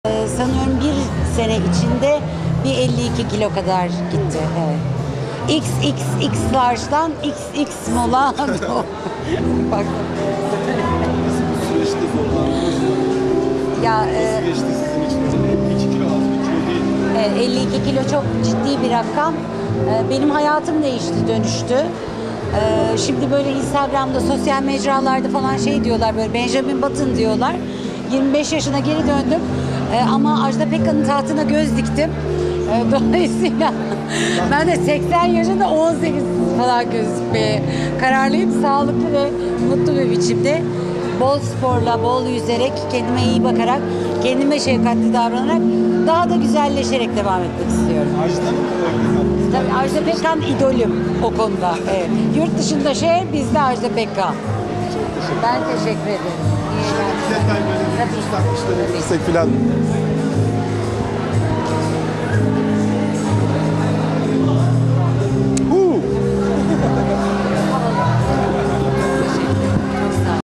52 HAFTADA 52 KİLO VEREN YEŞİM CEREN BOZOĞLU HAVASINI BÖYLE ATMIŞTI
Oyunculuğuyla göz dolduran Yeşim Ceren Bozoğlu ne kadar azimli olduğunu kısa sayılacak bir sürede verdiği kilolarla da eşe dosta gösterdi. Bozoğlu’nun haklı gurur, mutluluk ve sevincini haykırdığı o röportajını unutmak ne mümkün.

YEŞİM CEREN BOZOĞLU
52 HAFTADA 52 KİLO VERDİ
AJDA PEKKAN